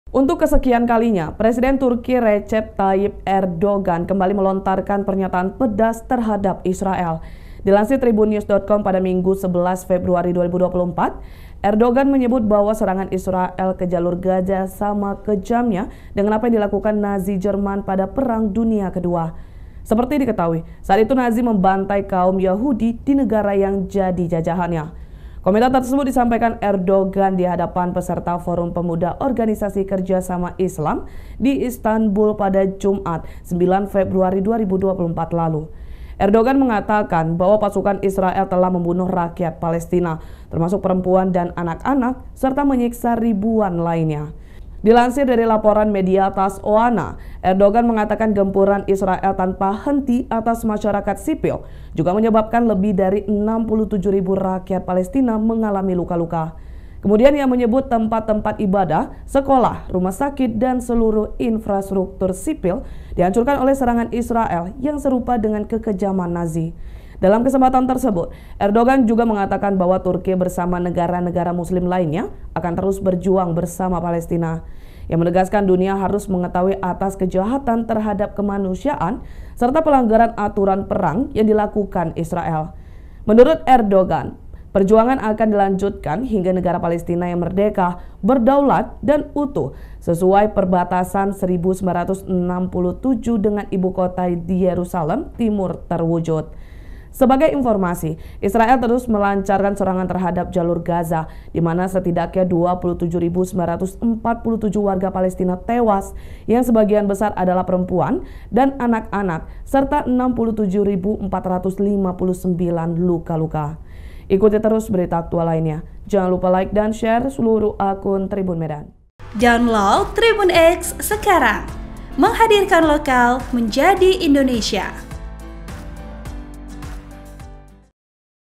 PRESIDEN TURKI Erdogan Marah Besar, Sebut Serangan Israel di Gaza Mirip Kekejaman Nazi di PD II!
TRIBUN-MEDAN.COM - Untuk kesekian kalinya, Presiden Turki, Recep Tayyip Erdogan kembali melontarkan pernyataan pedas terhadap Israel.

Dilansir Tribunnews.com pada Minggu (11/2/2024) Erdogan menyebut, bahwa serangan Israel ke Jalur Gaza sama kejamnya dengan apa yang dilakukan Nazi Jerman pada Perang Dunia II.

Seperti diketahui, saat itu Nazi membantai kaum Yahudi di negara yang jadi jajahannya.

Komentar tersebut, disampaikan Erdogan di hadapan peserta forum pemuda Organisasi Kerjasama Islam di Istanbul pada Jumat (9/2/2024) lalu.

Erdogan, mengatakan bahwa pasukan Israel telah membunuh rakyat Palestina, termasuk perempuan dan anak-anak, serta menyiksa ribuan lainnya.

Dilansir dari laporan media TASS-OANA, Erdogan mengatakan gempuran Israel tanpa henti atas masyarakat sipil juga menyebabkan lebih dari 67 ribu rakyat Palestina mengalami luka-luka.

Kemudian, ia menyebut, tempat-tempat ibadah, sekolah, rumah sakit dan seluruh infrastruktur sipil dihancurkan oleh serangan Israel yang serupa dengan kekejaman Nazi.

"Gempuran Israel tanpa henti atas masyarakat sipil juga menyebabkan lebih dari 67 ribu rakyat Palestina mengalami luka-luka," kata dia dikutip dari TASS-OANA.

"Tempat-tempat ibadah, sekolah, rumah sakit, dan seluruh infrastruktur sipil dihancurkan oleh serangan Israel yang serupa dengan kekejaman Nazi," ucap Erdogan.

Dalam kesempatan tersebut, Erdogan juga mengatakan bahwa Turki bersama negara-negara muslim lainnya akan terus berjuang bersama Palestina.

Ia menegaskan, dunia harus mengetahui atas kejahatan terhadap kemanusiaan serta pelanggaran aturan perang yang dilakukan Israel.

Menurut Erdogan, perjuangan akan dilanjutkan hingga negara Palestina yang merdeka, berdaulat dan utuh sesuai perbatasan 1967 dengan ibu kota di Yerusalem Timur terwujud.

"Kami terus berupaya melalui jalur diplomasi untuk memastikan negara-negara Islam bertindak serentak terhadap penindasan Israel di Jalur Gaza," lanjutnya.

"Kami akan melanjutkan perjuangan hingga negara Palestina yang merdeka, berdaulat, dan utuh sesuai perbatasan 1967 dengan ibu kota di Yerusalem Timur terwujud," tegas Erdogan.

Sementara itu, Menteri Luar Negeri Turki, Hakan Fidan mengkritik keheningan komunitas internasional terhadap pembantaian rakyat Palestina di Gaza.

Fidan menyebutnya sebagai tanda keruntuhan hukum internasional. Ia juga menyerukan untuk memperkuat tekanan diplomatik terhadap Israel.

Fidan menekankan, bahwa konflik di Jalur Gaza bukanlah masalah lokal semata, karena dapat memiliki dampak global jika pertempuran terus berlanjut dan solusi dua negara antara Israel dan Palestina tidak tercapai.

Sebagai informasi, Israel terus melancarkan serangan terhadap Jalur Gaza, di mana setidaknya 27.947 warga Palestina tewas yang sebagian besar adalah perempuan dan anak-anak, serta 67.459 luka-luka.

Israel telah menyerbu Jalur Gaza sejak serangan lintas batas oleh Hamas pada 7 Oktober 2023 laku, yang menurut Tel Aviv telah menewaskan hampir 1.200 orang.

Serangan Israel tersebut menyebabkan 85 persen dari populasi Gaza mengungsi secara internal di tengah kekurangan makanan, air bersih, dan obat-obatan yang akut, sementara 60 persen dari infrastruktur enklaf tersebut rusak atau hancur, menurut PBB.